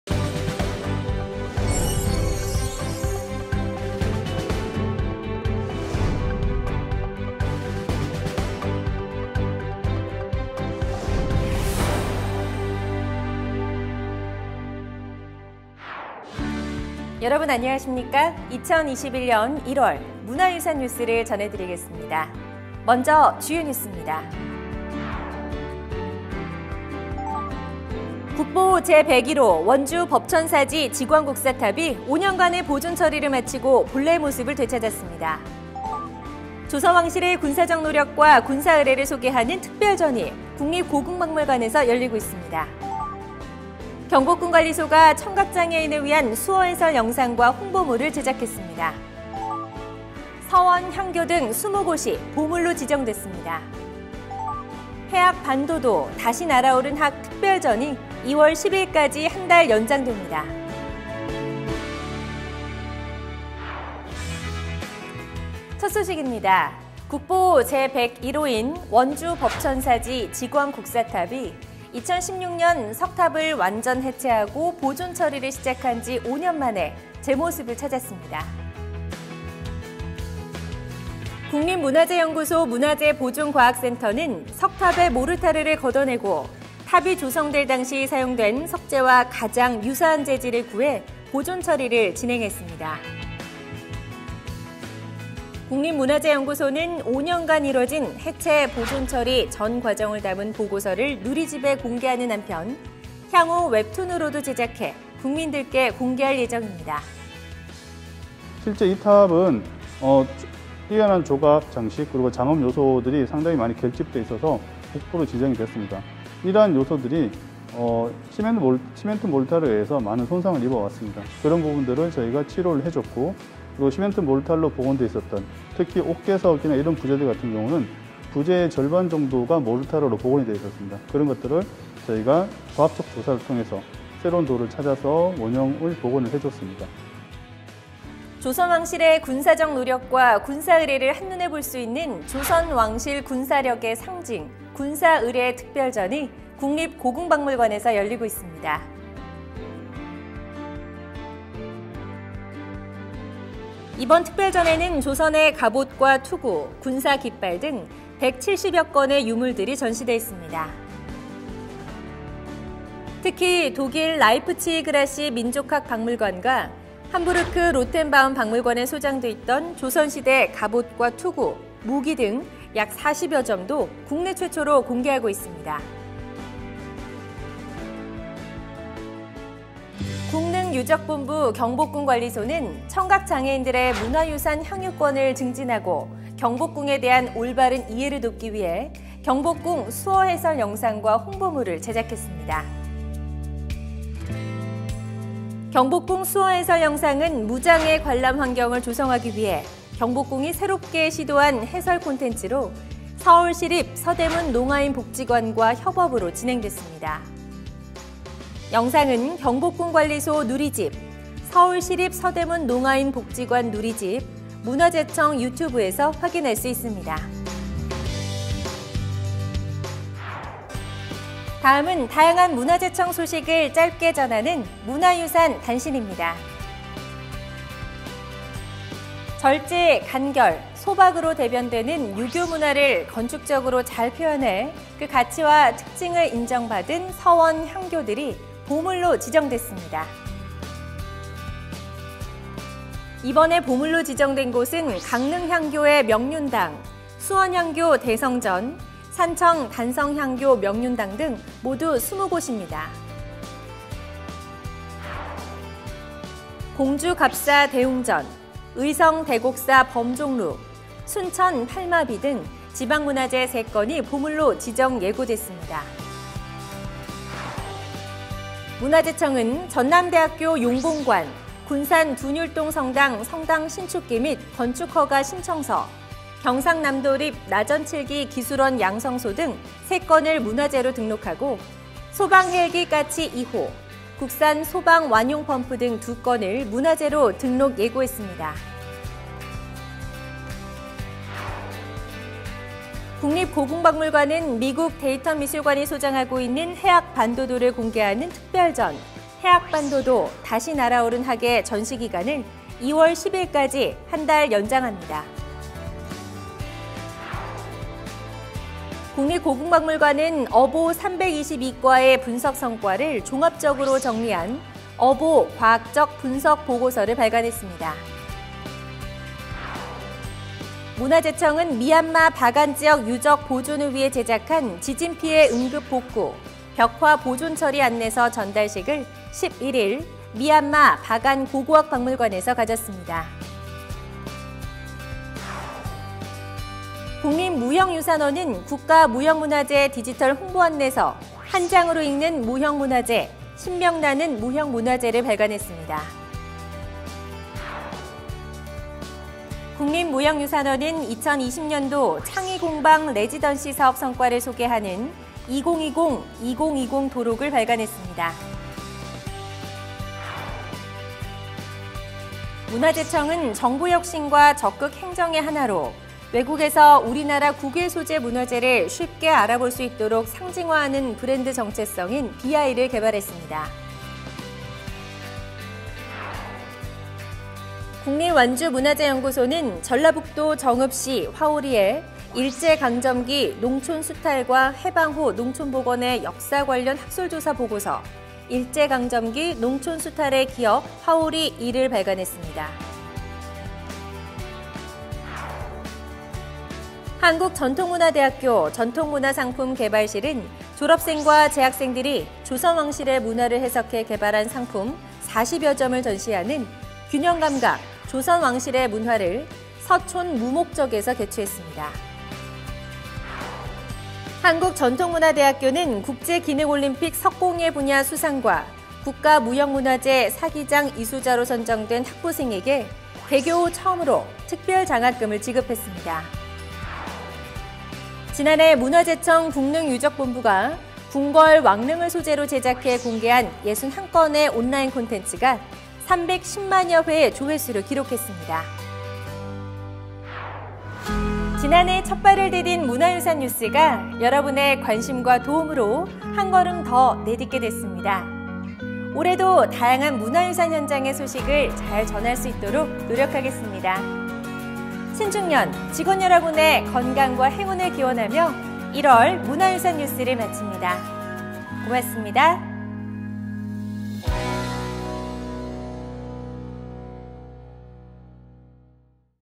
문화유산뉴스 (2021년 1월)
01:06 - 지광국사탑 완전 해체 후 5년만에 제모습 찾아
02:18 - 조선 왕실 군사력의 상징, 군사의례 특별전 열려
03:05 - 경복궁 수어해설 영상과 홍보물 제작
03:52 - 유교문화를 건축적으로 잘 표현한 서원, 향교 보물지정
04:05 - 강릉향교의 명륜당 등 20곳 보물 지정
04:17 - 공주 갑사 대웅전, 의성 대곡사 범종루, 순천 팔마비 3건 보물로 지정
04:34 - '전남대학교 용봉관' 등 문화재로 등록
04:40 - 소방헬기 까치 2호, 국산 소방 완용 펌프 문화재 등록 예고
04:52 - 해학 반도도, 다시 날아오른 학, 전시 2월 10일까지 연장
05:08 - '어보 과학적 분석' 보고서 발간
05:21 - 대한민국 벽화 보존관리 기술 안내서 미얀마에 전달
05:37 - '한장으로 읽는 무형문화재 - 신명 나는 무형문화재' 발간
05:51 - '2020 이공이공' 도록 발간
06:02 - 국외소재문화재 BI 개발
06:20 - 국립완주문화재연구소 '일제강점기 농촌 수탈의 기억 화호리' 발간
06:38 - 한국전통문화대학교 - 졸업생과 재학생들이 개발한 상품 40여 점을 전시
06:49 - '균형 감각, 조선 왕실의 문화' 특별전 개최
06:57 -  한국전통문화대학교, 개교후 처음으로 특별 장학금 지급
07:14 - 궁궐, 왕릉 소재로 제작된 온라인 콘텐츠 310만여 회 조회수 기록